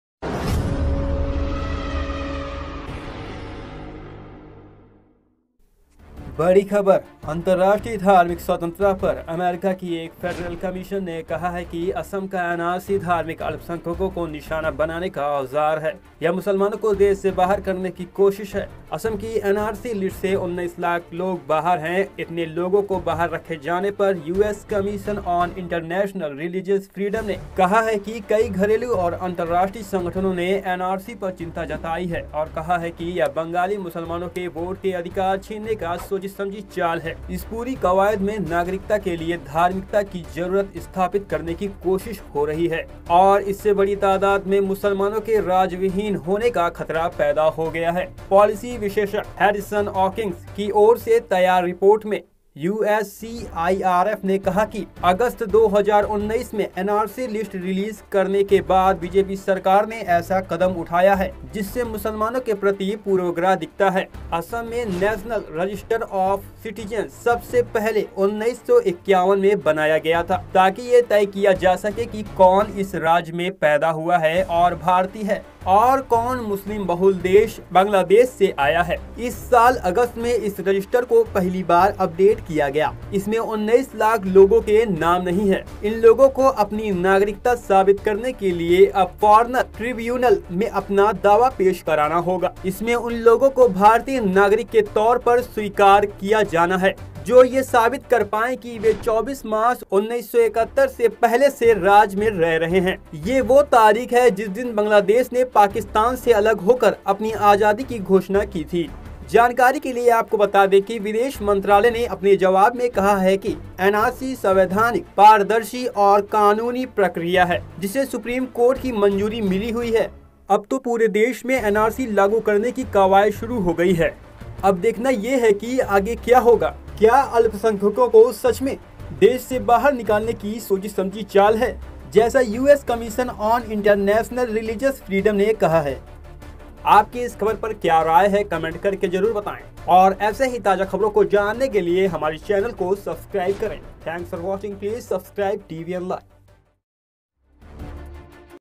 The US Commission on International Religious Freedom (USCIRF) give a big Statement on NRC India..
अंतरराष्ट्रीय धार्मिक स्वंत्रता पर अमेरिका की एक फेडरल कमीशन ने कहा है कि असम का एनआरसी धार्मिक अल्पसंख्यकों को निशाना बनाने का औजा-र है. यह मुस्लिमों को देश से बा-ह'र करने की कोशिश है..

 A US Federal Commission on International Religious Freedom has said that the NRC of Assam is a tool to target Religious Minorities..